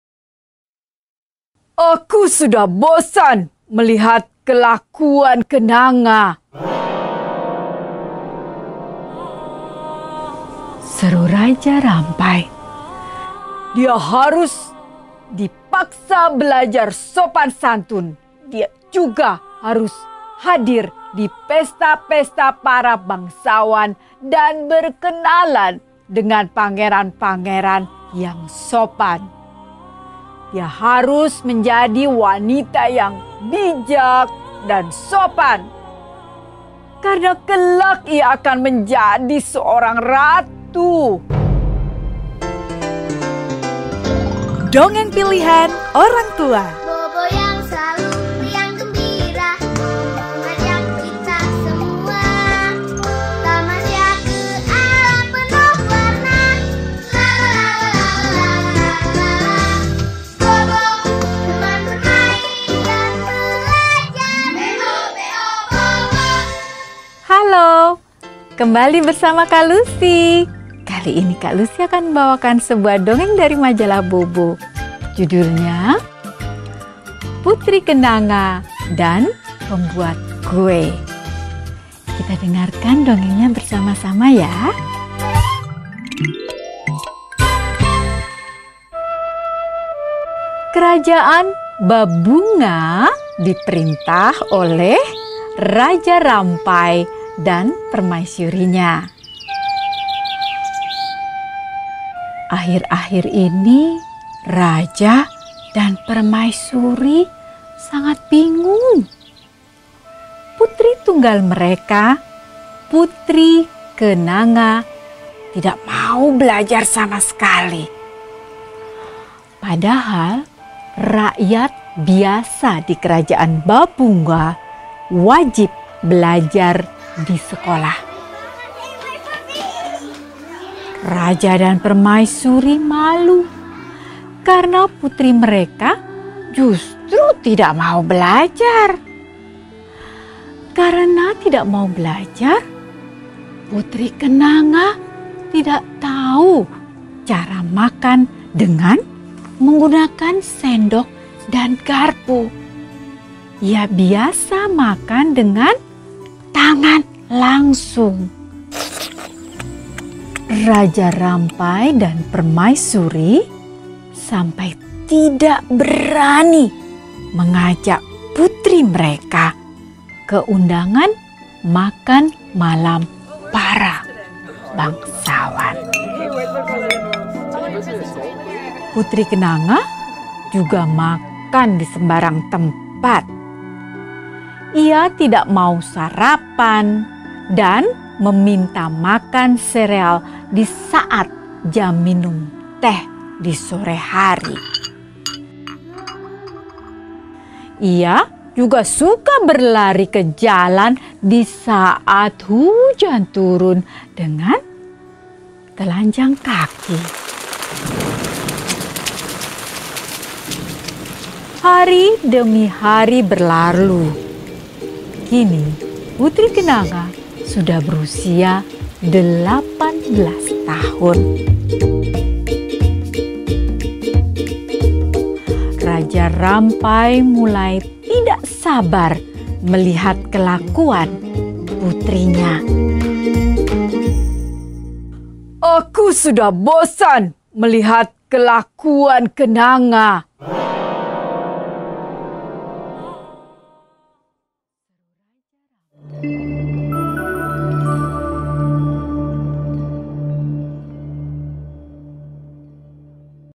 Dongeng Putri Kenanga dan Pembuat Kue | Dongeng Pilihan Orangtua
Kerajaan Babungan di perintah oleh Raja Rampai dan permaisurinya. Akhir-akhir ini Raja dan permaisuri sangat bingung karena Putri Kenanga tidak mau belajar sama sekali. Padahal rakyat biasa wajib belajar di sekolah. Raja dan permaisuri sangat malu karena Putri Kenanga tidak mau belajar. Hingga suatu hari, Putri Kenanga memutuskan tidak ingin menjadi putri lagi. Bagaimana Raja dan permaisuri menanggapinya, ya?